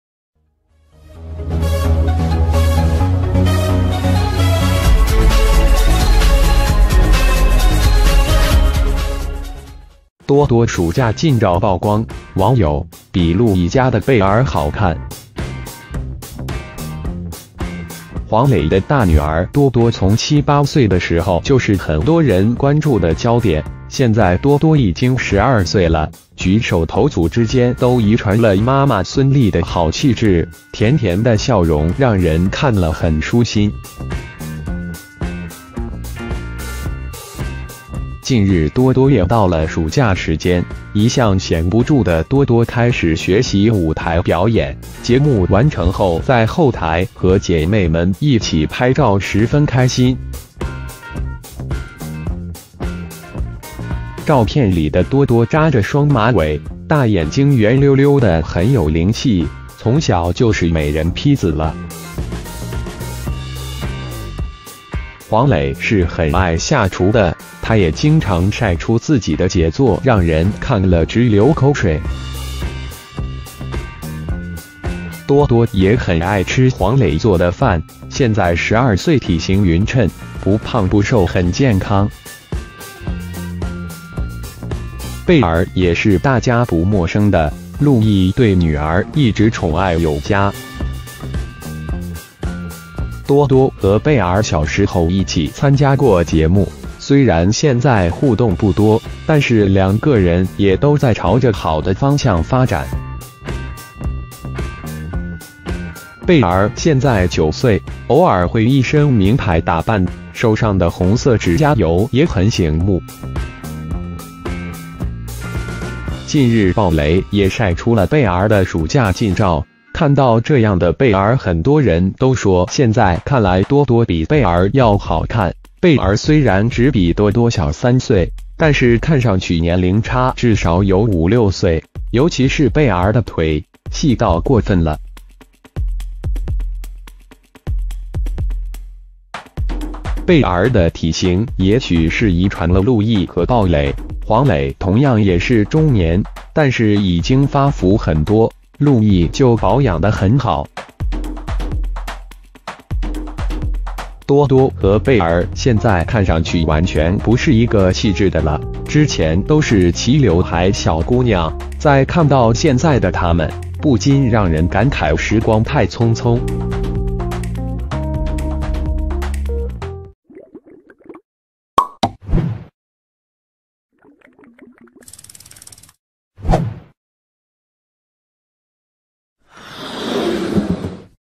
多多暑假近照曝光，网友：比陆毅家的贝儿好看！ ✔
✔ MC Network [ 36 寵物 ] 多多暑假近照曝光，网友：比陆毅家的贝儿好看！
黄磊的大女儿多多从七八岁的时候就是很多人关注的焦点，现在多多已经12岁了，举手投足之间都遗传了妈妈孙莉的好气质，甜甜的笑容让人看了很舒心。
近日多多也到了暑假时间，一向闲不住的多多开始学习舞台表演，节目完成后在后台和姐妹们一起拍照十分开心。照片里的多多扎着双马尾，大眼睛圆溜溜的很有灵气，从小就是美人坯子了。